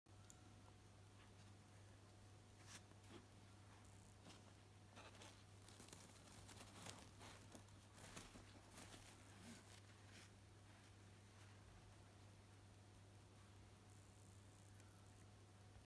Realistic toy/Rat/Mouse
Manual rat original size.
Color options.
A hand rat is a little friend. Repeats all shapes and sizes of the original animal.